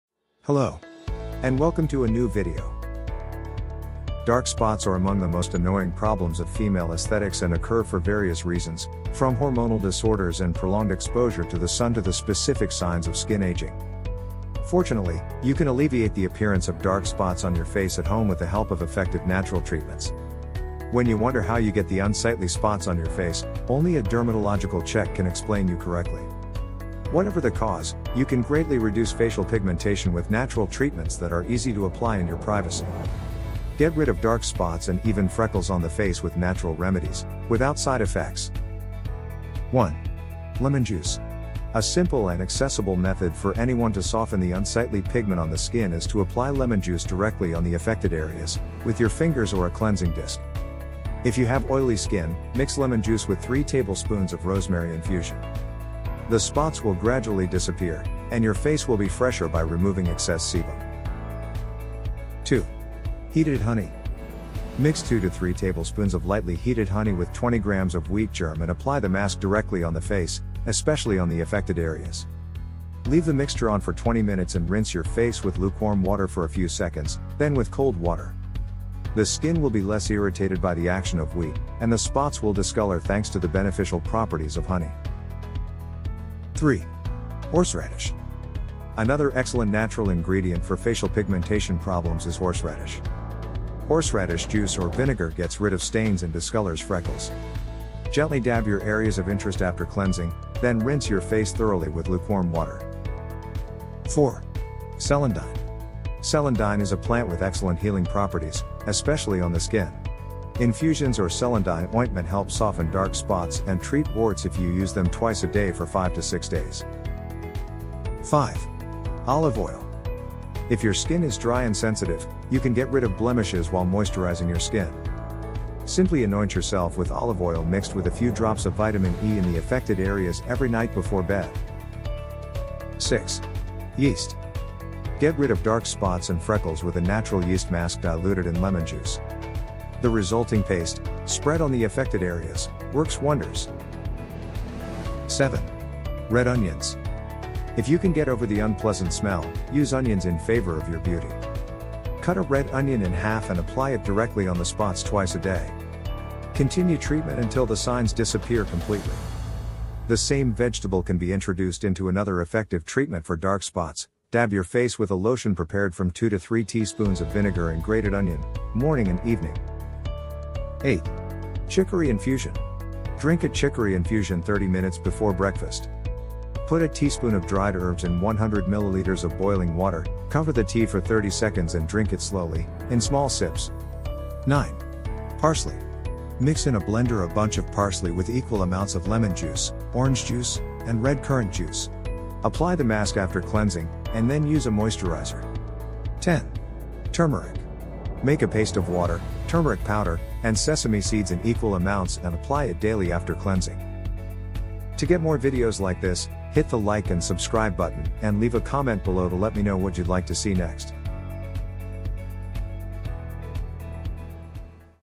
✔️ 10 Natural Treatments that Get Rid of DARK SPOTS from Your Face
10 natural treatments that get rid of dark spots from your face

Dark spots are among the most annoying problems of female aesthetics and occur for various reasons, from hormonaldisorders and prolonged exposure to the sun to the specific signs of skinaging. Fortunately, you can alleviate the appearance of dark spots on your face at home with the help of effective natural treatments.

When you wonder how you get the unsightly spots on your face, only a dermatological check can explain you correctly. Whatever the cause, you can greatly reduce facial pigmentation with natural treatments that are easy to apply in your privacy. Get rid of dark spots and even freckles on the face with natural remedies, without side effects!

Photo by Ike louie Natividad from Pexels